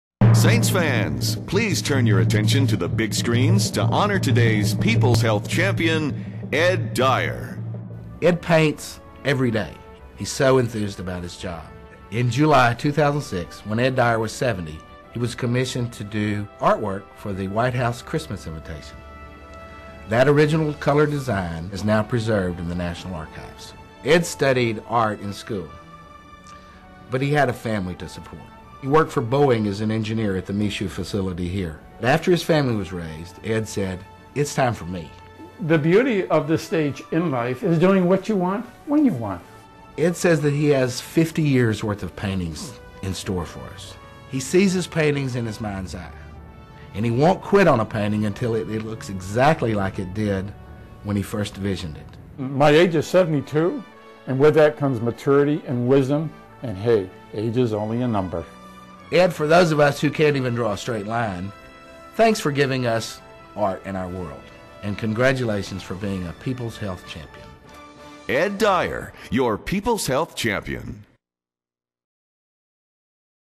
Ed Dyer - 2007 Peoples Health Champion
In July of 2006, at the age of 70, Ed Dyer was commissioned to design the 2006 White House Christmas Invitation. Ed’s original painting of a holiday scene at the White House graced the cover of the card and is now preserved as part of our nation’s history in the National Archives.

Ed’s White House commission is especially impressive considering he only started painting seriously six years earlier.

As a child and through school, Ed loved the arts and enjoyed painting as a hobby. His creativity led him to own an advertising agency and even play professional baseball before a leg injury steered him to be an engineer at Michoud for Boeing. After officially retiring in 1999, Ed decided that now, it was his chance to return to what he considered his true calling—painting.

According to Ed, his first painting was so bad he refuses to let anyone see it (yes, we asked…) – but he didn’t let that stop him. Supported by the confidence he built throughout his life from the different hats he wore in each experience, Ed objectively identified his mistakes and started on his second painting. He entered that painting in a competition judged by the editor of American Artist magazine. It won 1st prize. Since then he has sold numerous paintings and won multiple national art awards.

Isn’t it nice to know that life experience allows us the opportunity to grow, to learn from our mistakes and to improve? Life experience is the foundation for success.

Ed Dyer’s days are filled with doing what he loves. He wakes up each morning eager to continue the painting he stopped working on the night before. Ed ends each day with a smile on his face, looking forward to the next. Living life to the fullest.